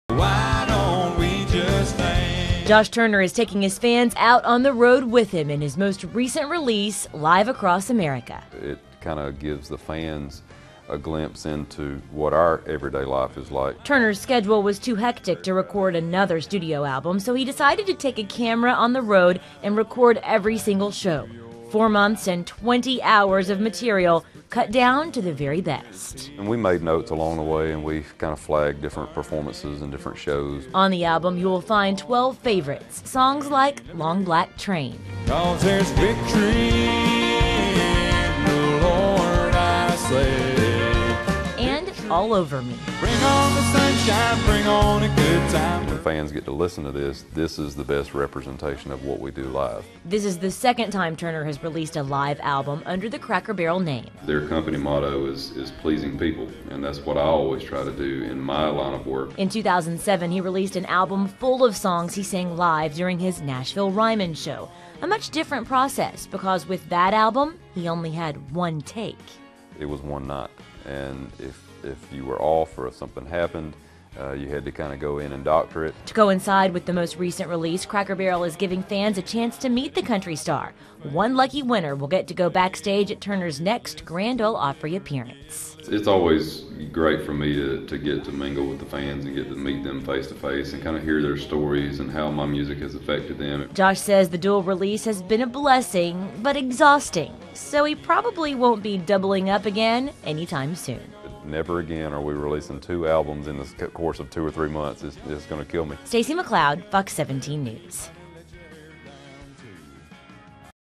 OMG One of the deepest voice I have ever heard in my life!!!!
Josh turner one hell of a deep voice
hello, this is a country music and pokemon mystery dungeon focused channel as of and Kirby anime 2022
so now I will be concentrating on getting videos that originally got a lot of views.